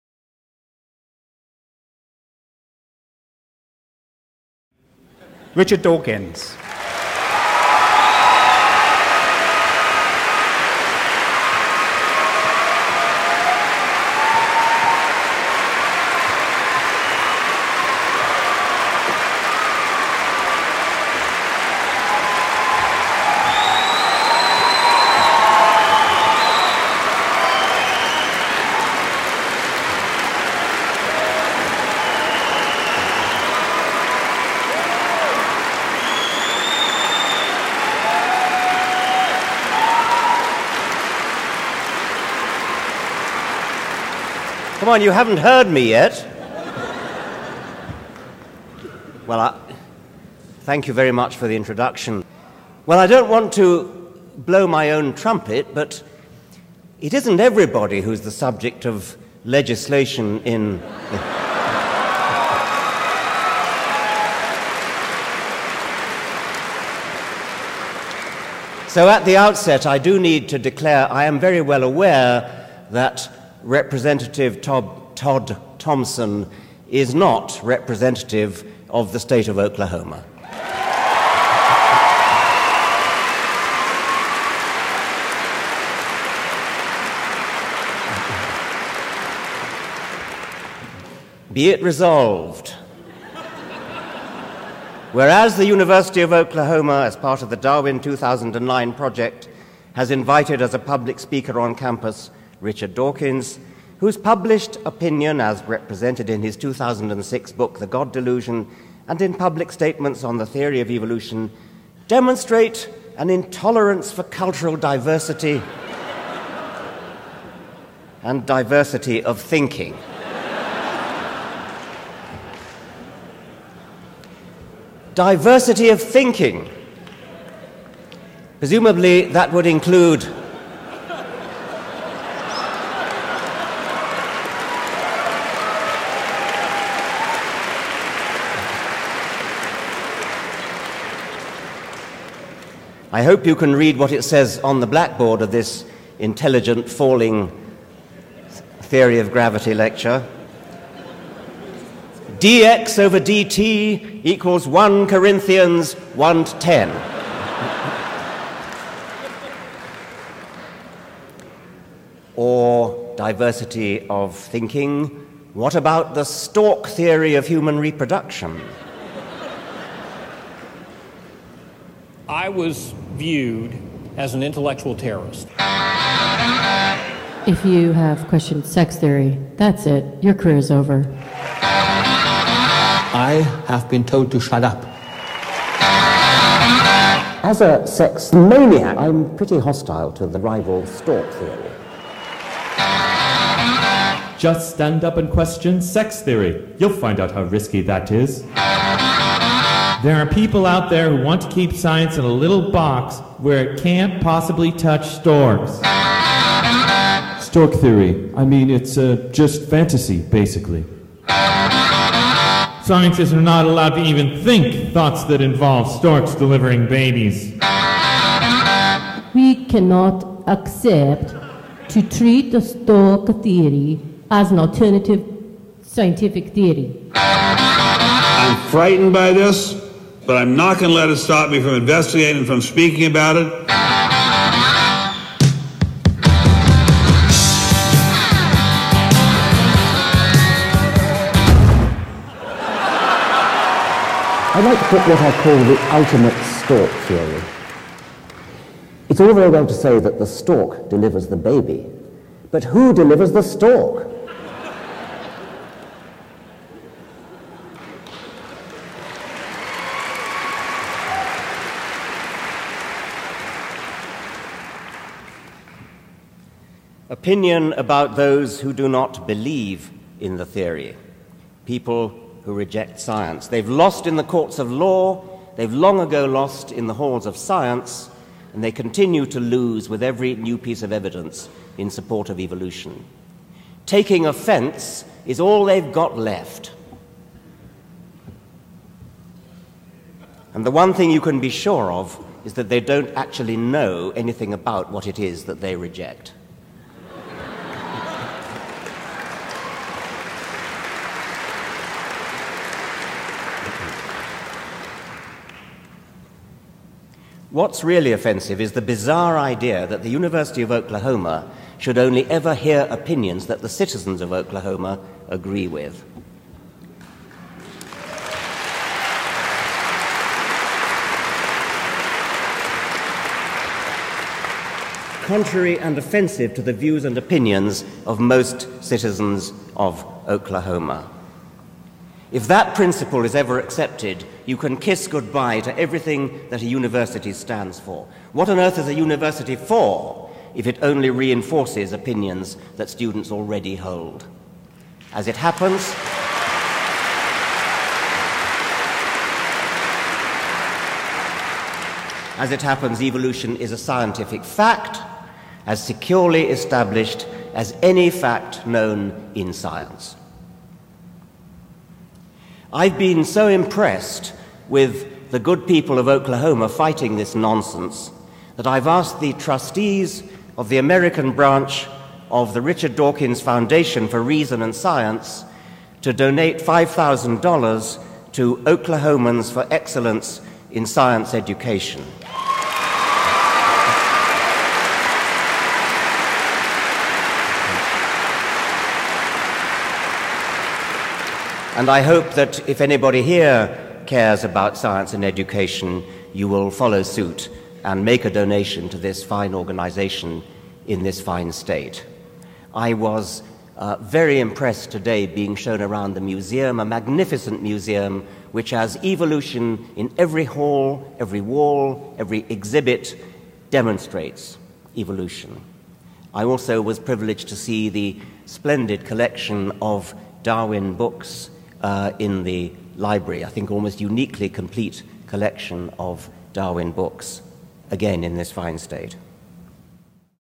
Richard Dawkins at the University of Oklahoma
'This is Richard Dawkins' special introduction to his lecture at the University of Oklahoma on March 6th, 2009. See more about the events surrounding his visit here: